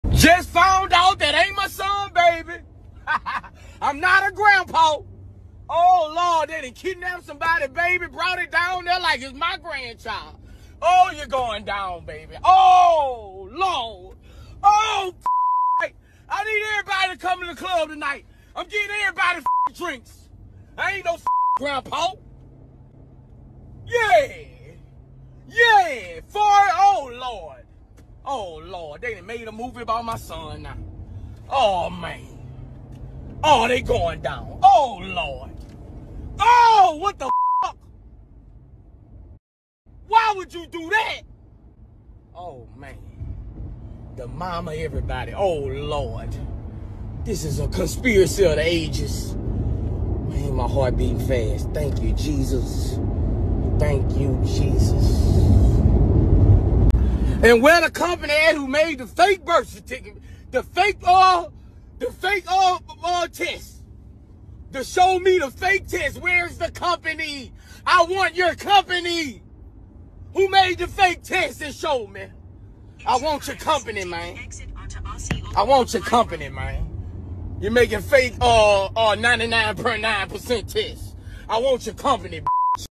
Lil Boosie Reacting To NOT BEING A GRANDFATHER + His Son Tootie Raw DNA Test Coming Back To Be Fake
IF YOU DONT KNOW HERE YOU GO! BOOSIE SO FUNNY BRO